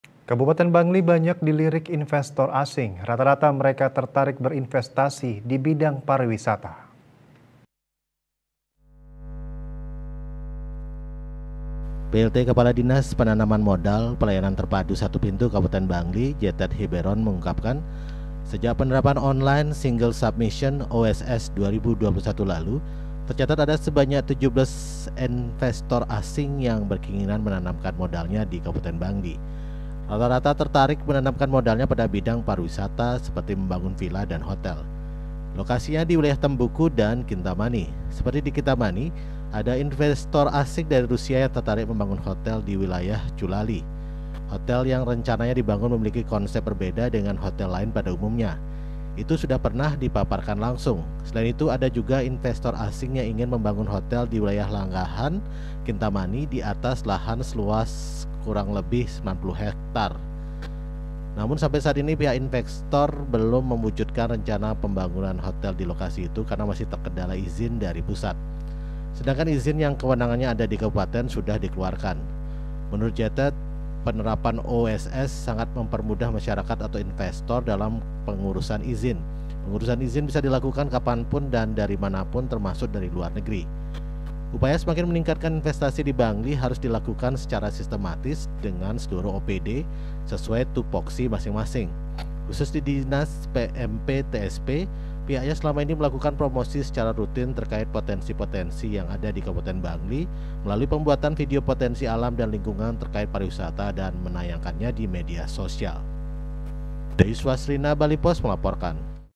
PMA DI BANGLI LEBIH TERTARIK SEKTOR PARIWISATA
KABUPATEN BANGLI BANYAK DILIRIK INVESTOR ASING. RATA-RATA MEREKA TERTARIK BERINVESTASI DI BIDANG PARIWISATA.